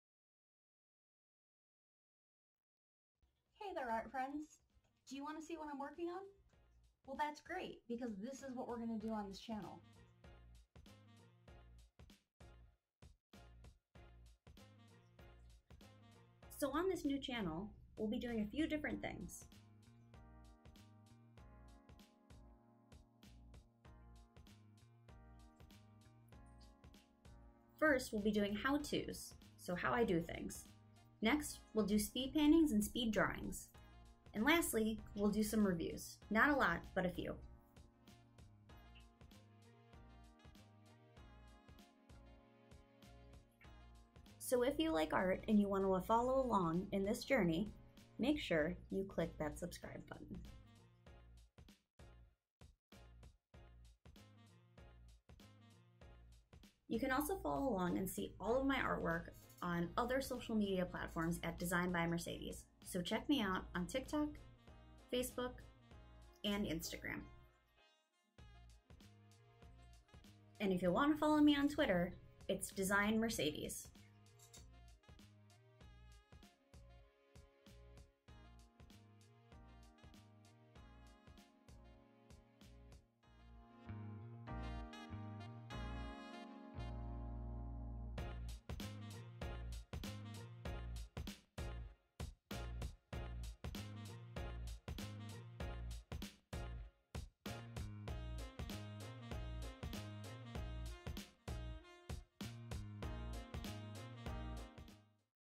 Hello Art Friends!
Hello Art Friends! My name is Mercedes and I love to arts.

On this channel I will be doing
-Speed paints/Watch me draws
-How-to's/How I do's
-Reviews on items